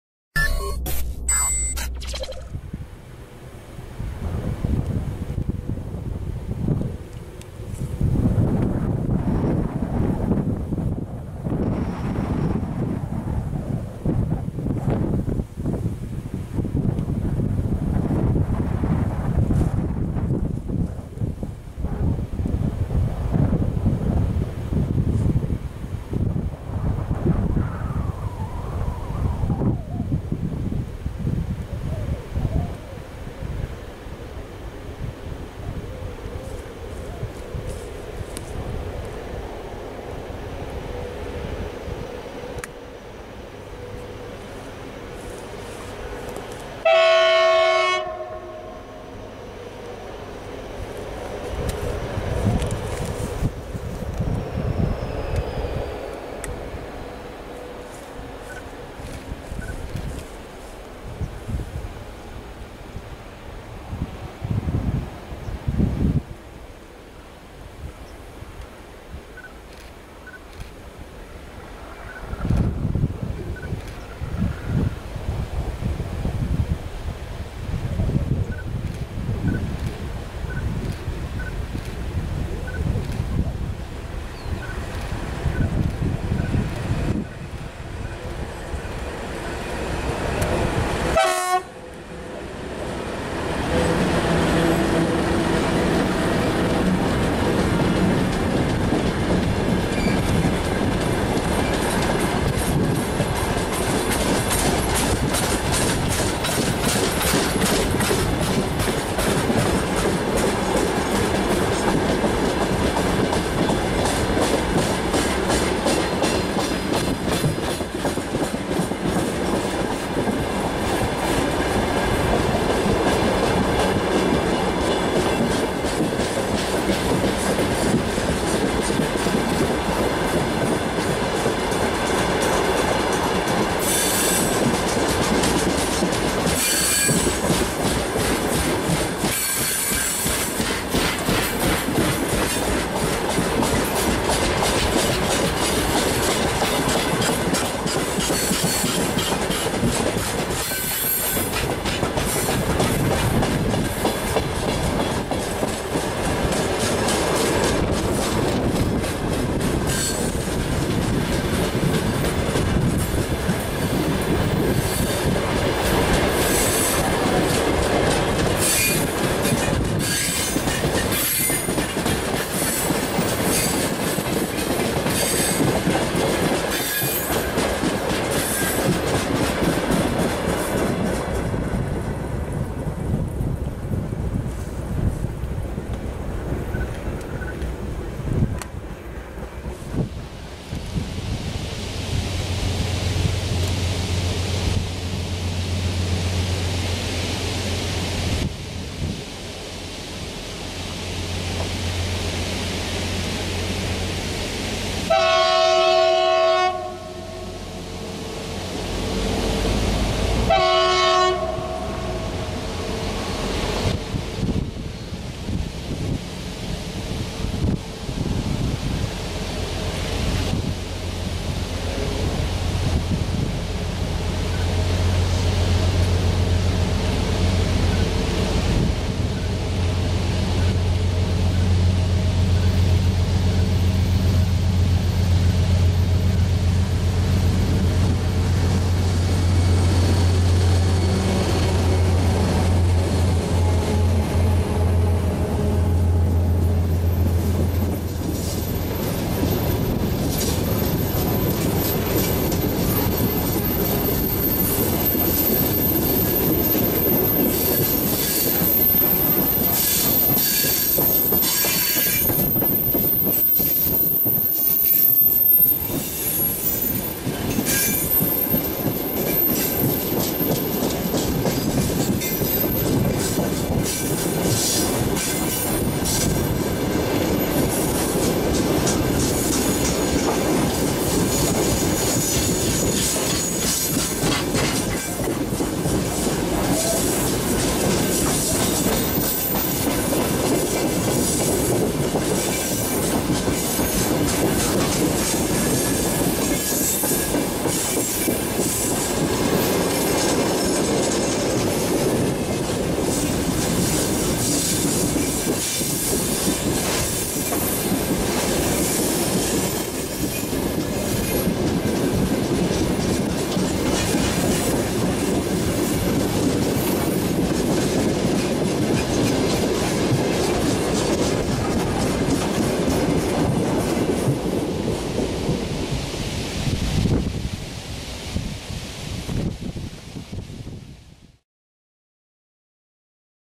LHS ST40-04 + ST40s-16 (Dwa Ujecia)
🇵🇱 Duet Batmanów (04 i 16)prowadzą składy rudy żelaza, docelowo do Sławkowa na Śląsku. Skład został nagrany na Kępiu, oraz w Wolbromiu (Małopolska) na linii nr.65 (LHS).

🇺🇸🇬🇧 A duo of LHS ST40s (Numbered 04 & 16) leads a loaded iron ore train to Sławków Poland. This train was recorded in two different locations: Kępie & Wolbrom, Poland on the LHS Railway (Line Nr.65).